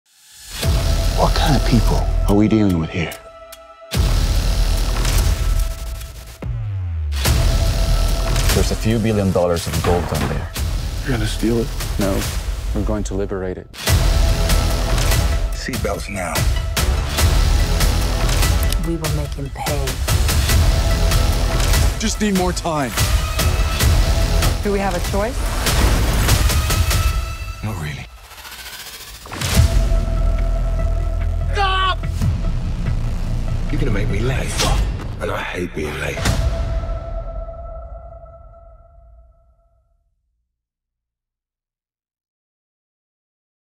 أهم الأفلام العالمية I أفلام الويكند I نهاية أسبوع مميزة
بين الجريـــمة، الإثـــارة، والأكشــــن... أفلام ستبقيك متسمراً أمام الشاشة في نهاية الأسبوع